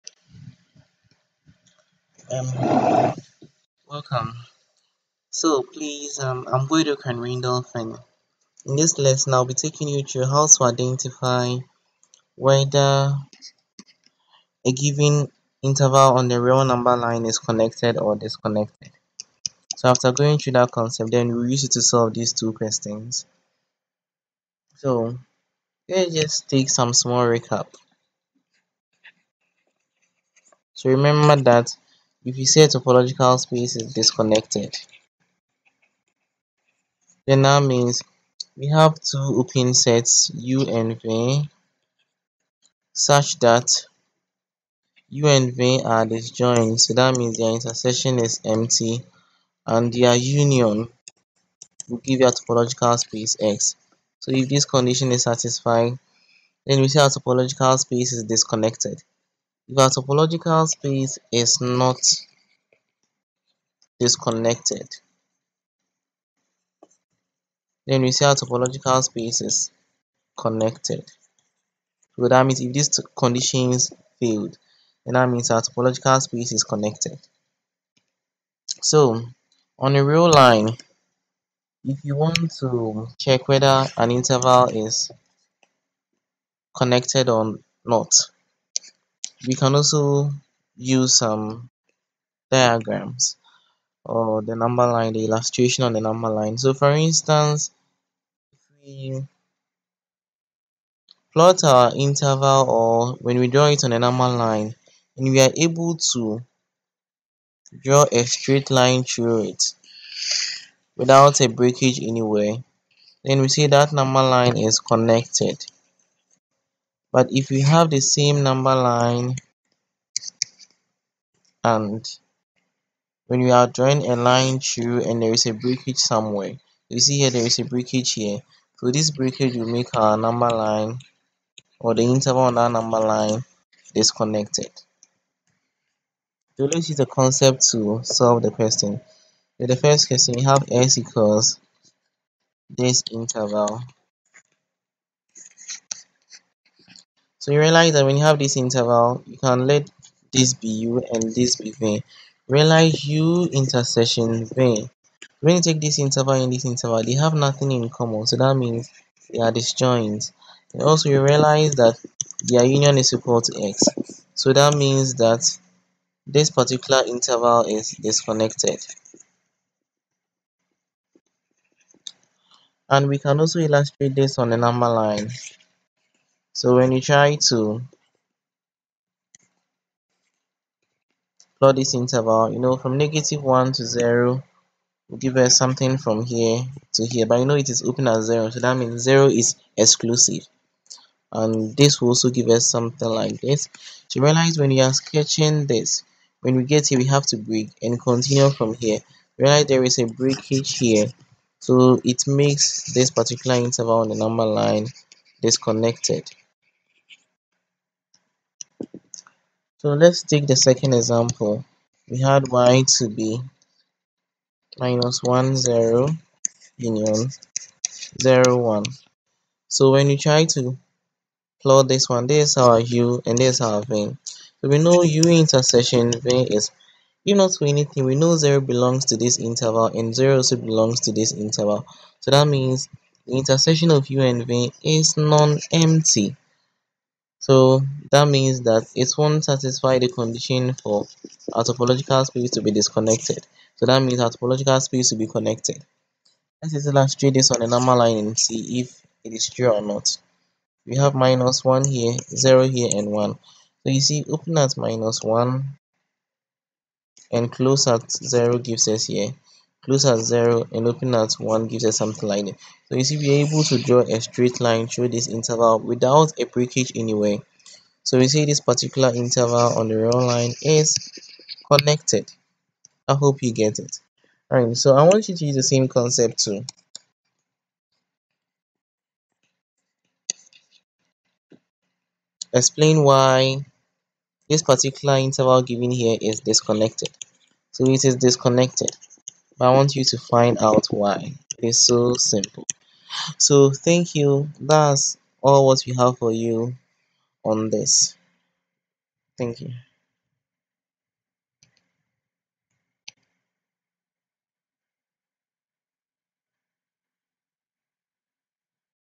How to identify whether an interval on the real line is connected or disconnected.
This helps us to determine whether an interval on the real line is connected or disconnected.